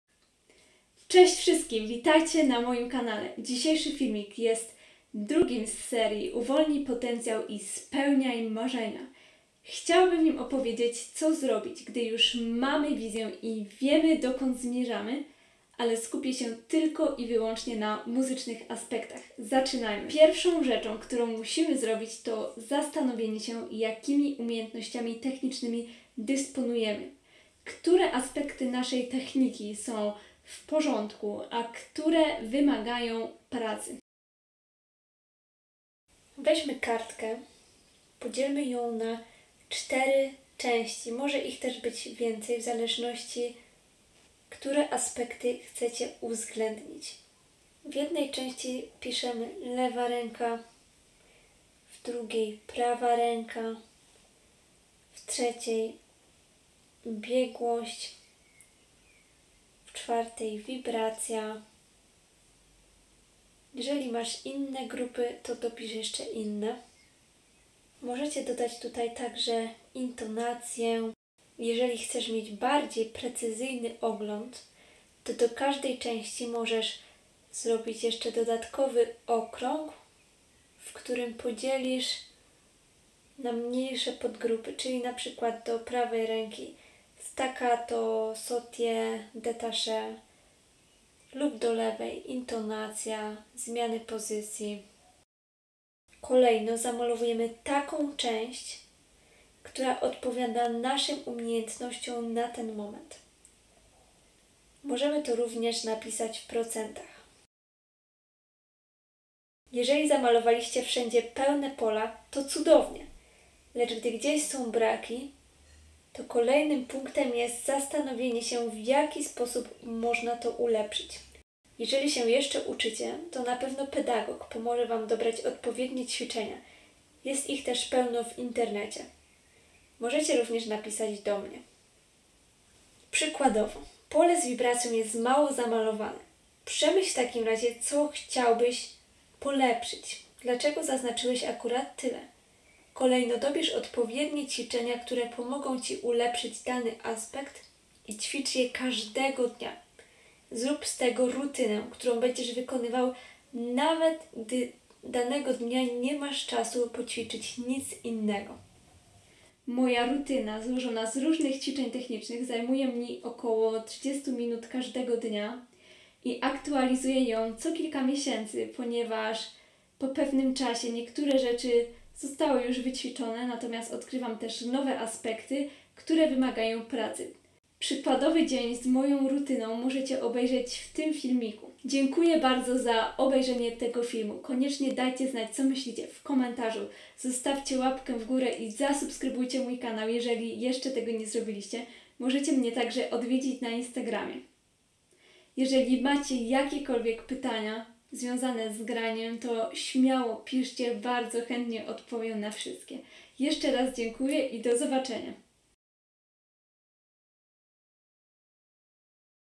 Co wymaga poprawy / Which technical aspect needs improvement? (ENG subtitles)
Hi! In this video I would like to talk about what to do next when you have the vision and you know where you want to be in the future, but I am going to focus only on the musical aspect.